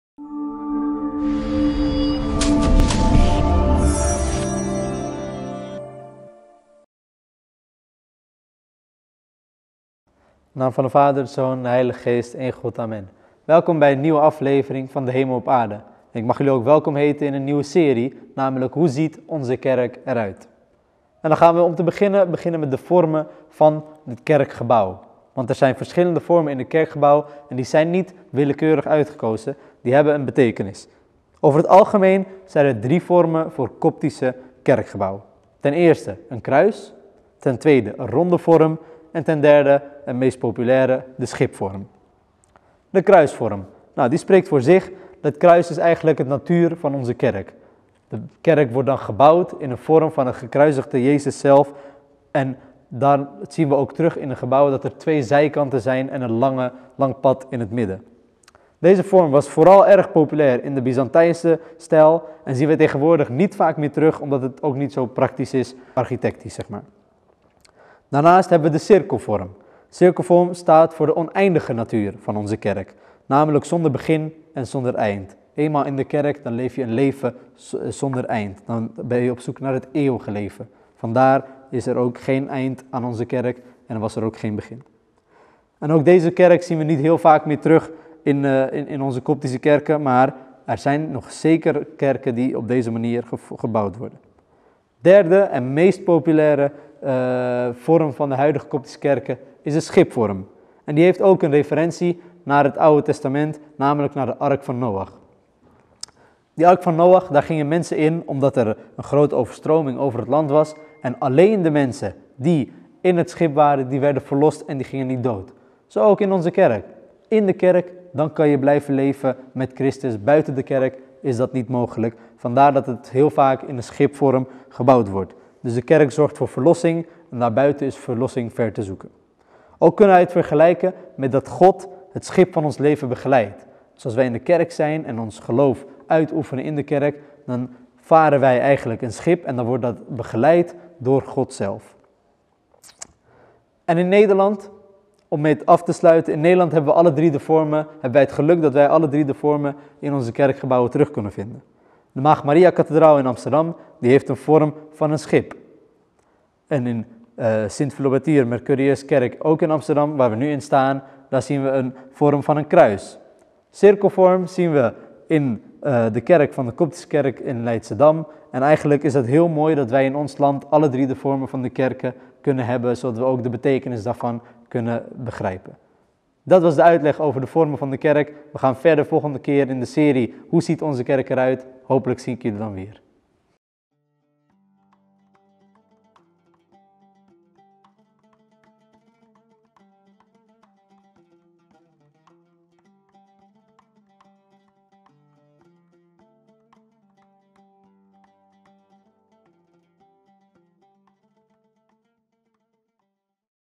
Hoe ziet de Kerk er uit? - Vormen van de Kerk - “De hemel op aarde”
“De hemel op aarde”
 Dienaar Kevin Kozman
“Aflevering 6-
Hoe ziet de kerk er uit? -  Vormen van de Kerk